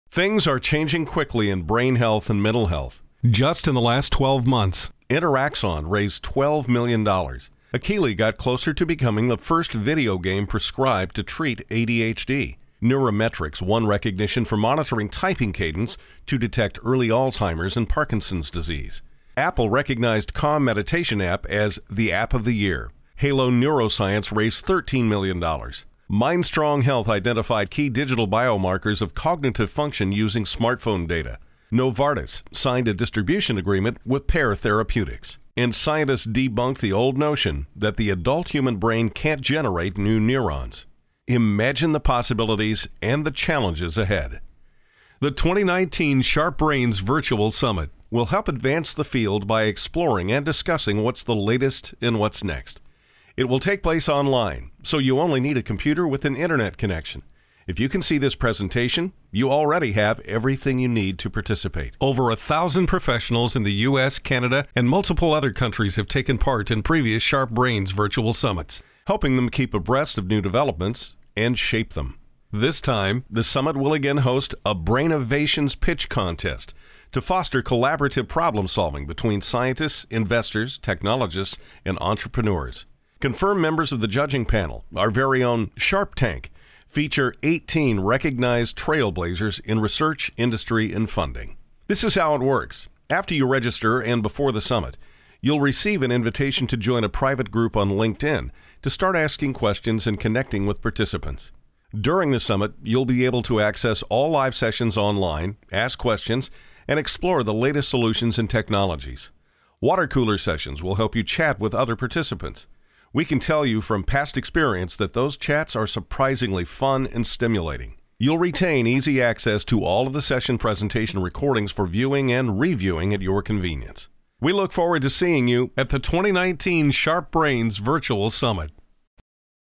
2019 SharpBrains Virtual Summit
Imagine your smartphone can detect early signs of anxiety, depression or Parkinson’s Disease — would you like to know? Who else should know … and not know?

Imagine a videogame cleared by the FDA to treat ADHD or substance abuse — how will doctors prescribe it, patients access it, and insurers pay for it?

Imagine being responsible for the health & wellness of your organization — how will you navigate the growing toolkit of evidence-based interventions such as exercise/ meditation/ sleep apps/ cognitive therapies/ biofeedback/ tDCS?

Imagine investing $100 million to help accelerate brain research and applications — how would you ensure that responsible development meets wise adoption?

Imagine a free annual brain check-up — who should take it, and how could it help prevent/ delay/ treat cognitive decline and Alzheimer’s Disease?

The 2019 SharpBrains Virtual Summit (May 7–9th, 2019)  will feature over forty of the world’s top experts, innovators and investors working to improve brain health for all in light of growing neuroscience and digital tech.